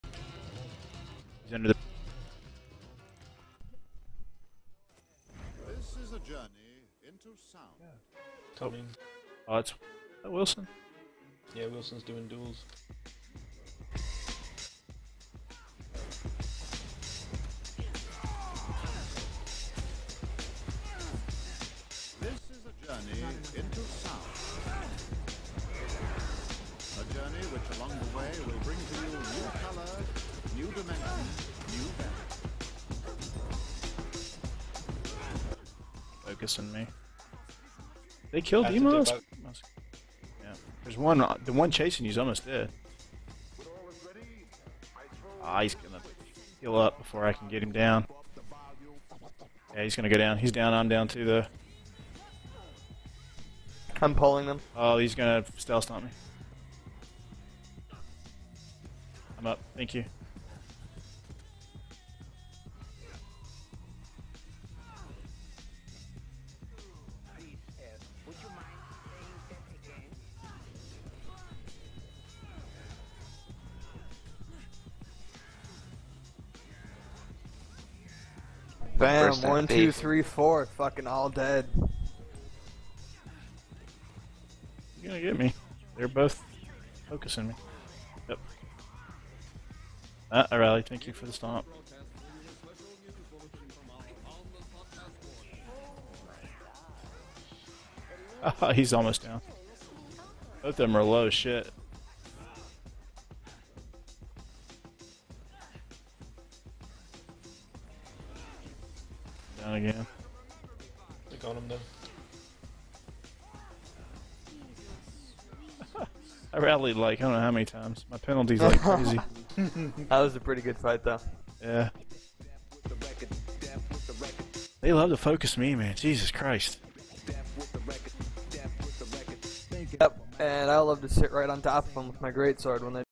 [Twitch] Episode 66 - Pro Strike Force
A nice skirmish with Pro and Strike Force at the lake, near their tower :)